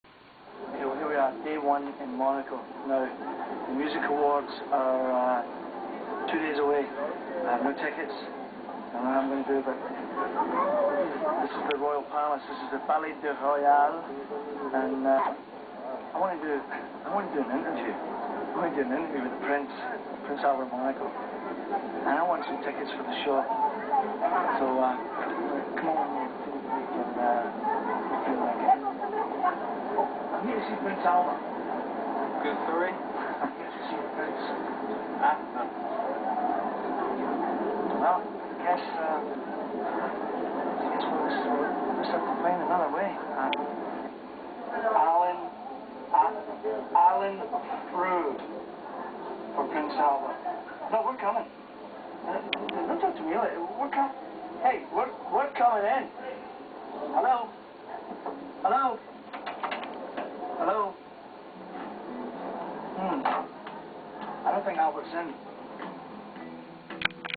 Road Stories with Alan Frew
Alan tries to get in to see Prince Albert